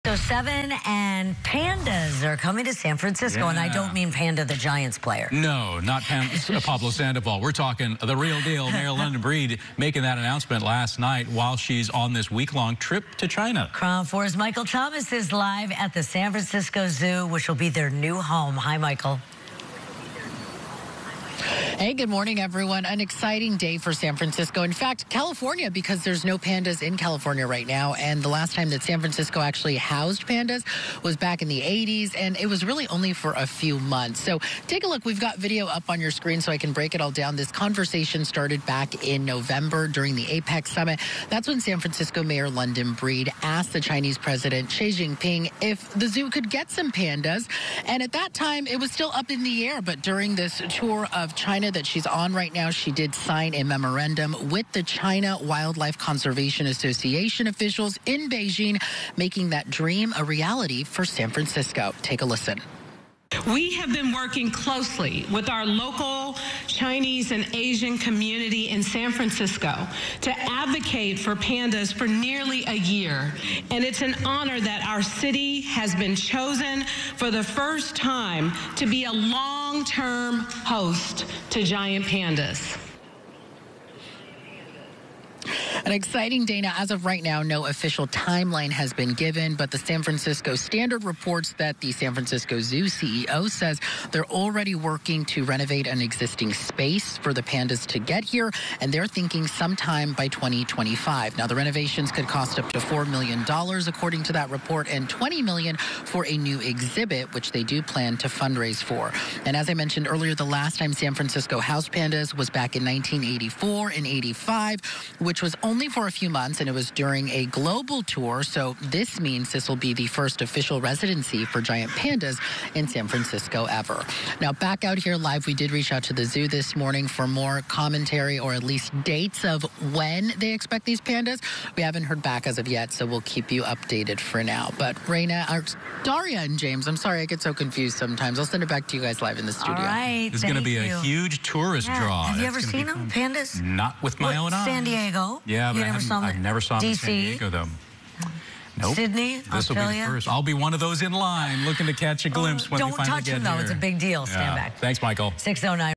San Francisco to get giant pandas from China
KRON4's Michael Thomas reports